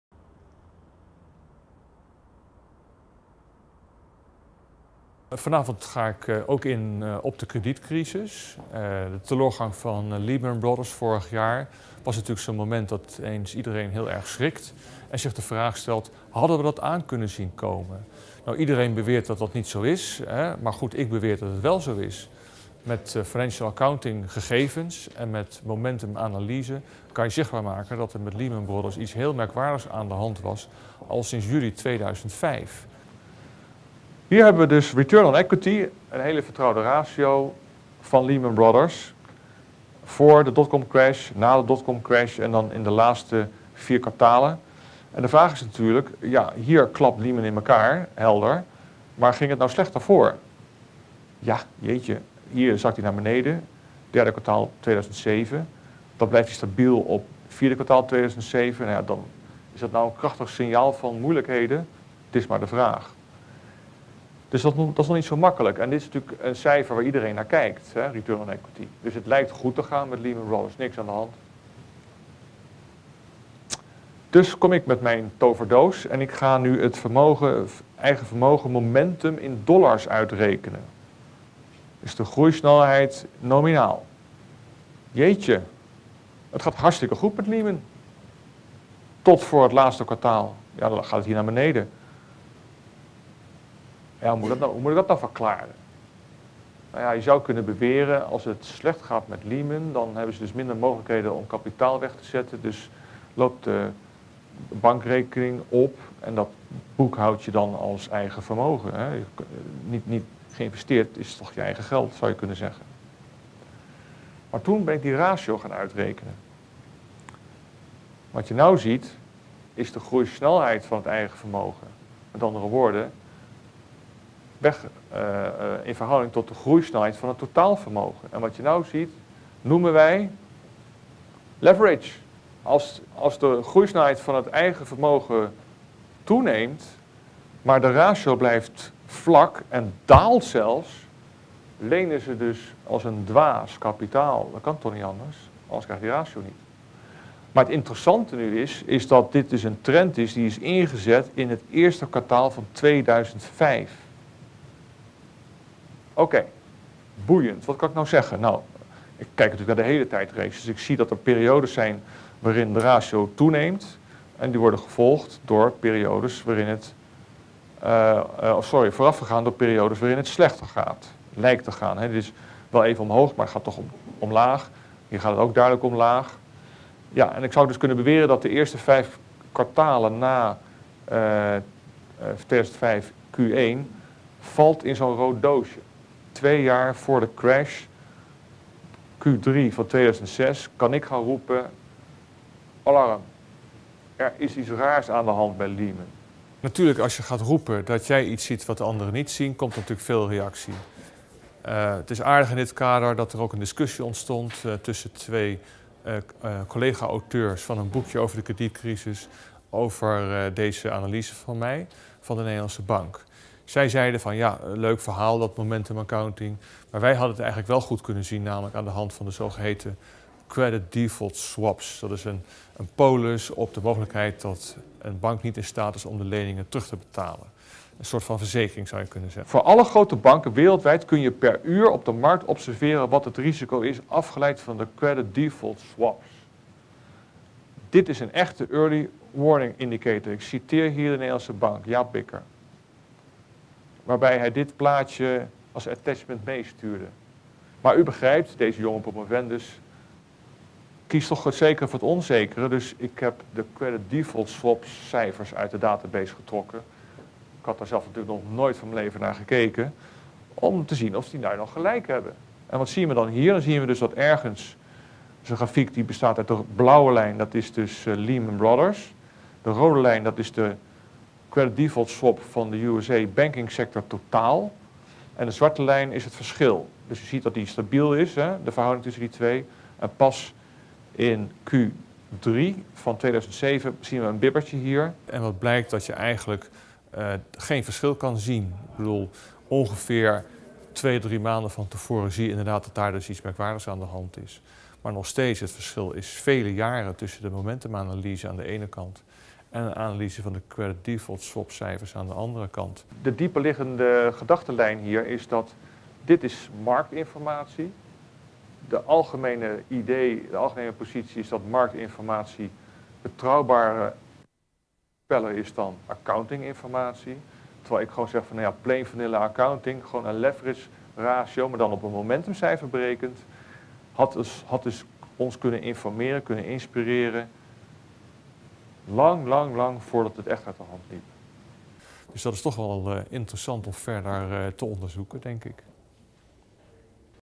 Lehman Brothers Case - Conquaestor Master Class
Watch a short Dutch language introduction by Eric Melse about Lehman Brothers and how with Triple Entry and Momentum Accounting-TEMA in short-the dynamic accounting theory of Yuji Ijiri, we could have known that this firm was already for three years having problems, during a Master Class at Conquaestor BV, the Dutch staffing firm of financial professionals.